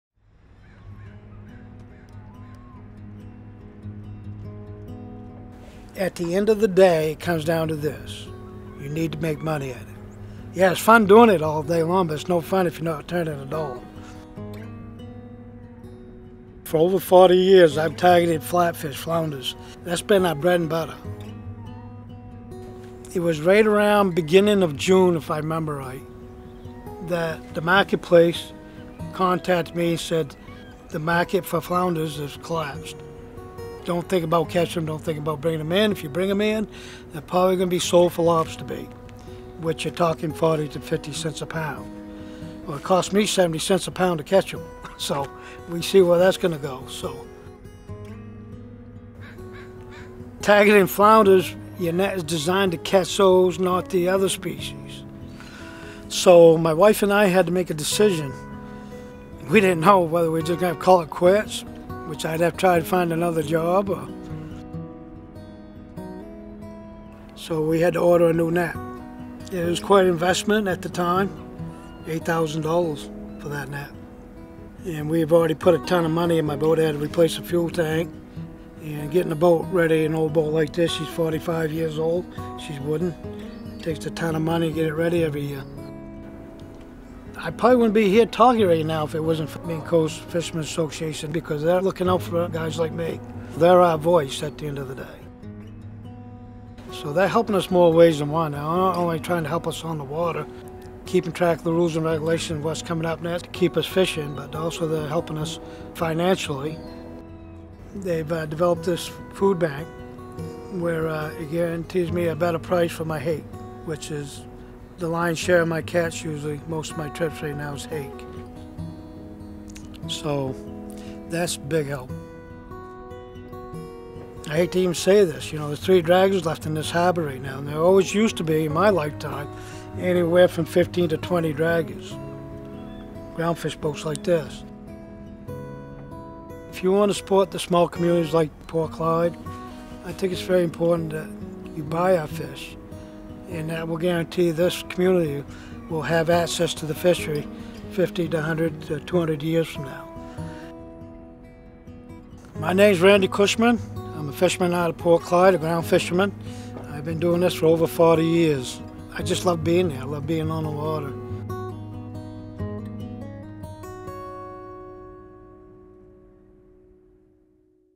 Randy Cushman. Groundfishing in Port Clyde, Maine.
Maine Coast Fishermen's Association presents Randy Cushman, a forth generation fisherman from Port Clyde Maine. Randy shares his story of perseverance as he strives to keep his fishing business going in response to COVID-19 while building a sustainable fishery for the future.

"I probably wouldn't be here talking to you right now if it wasn't for the Maine Coast Fishermen's Association because they're looking out for guys like me."

Created by Knack Factory